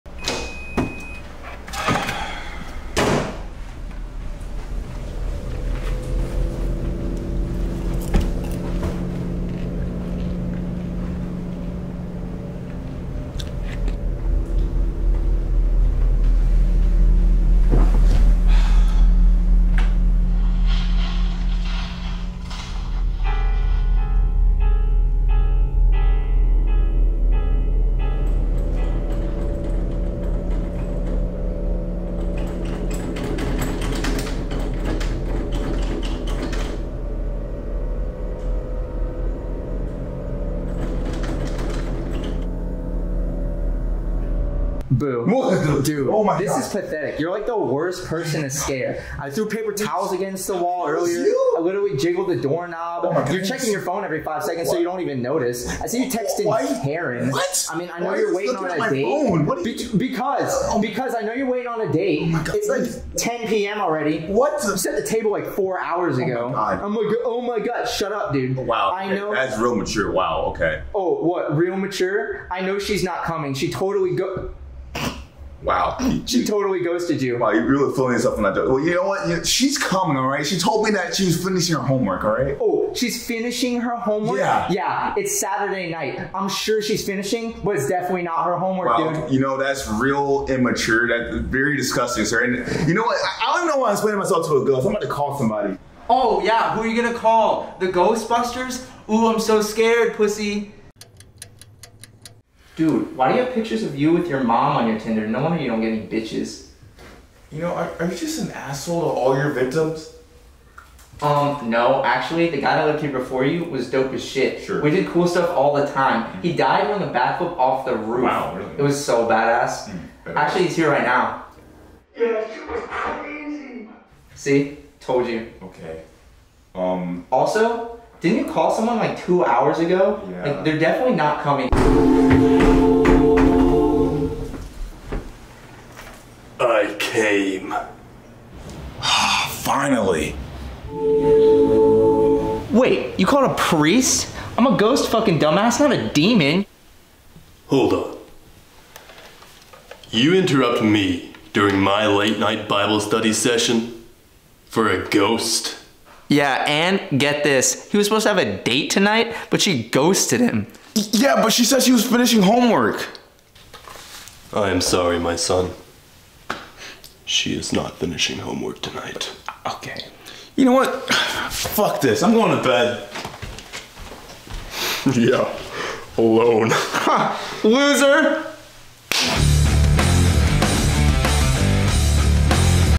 Ghost Roast | Halloween Short Film
Karen's such a BITCH! Happy Halloween, everyone!

Cast:
- Main Ghost: Alex Riese
- Kyle (victim): Perry Hopper
- Priest: Aidan Hubbs
- Other Ghost: Sam Langenfeld

Crew:
- Director: Sam Langenfeld
- Cinematographer: Sam Langenfeld
- Audio: Aidan Hubbs
- Producer: Sam Langenfeld
- Writer(s): Sam Langenfeld, Alex Riese, Perry Hopper, Aidan Hubbs
- Editor: Sam Langenfeld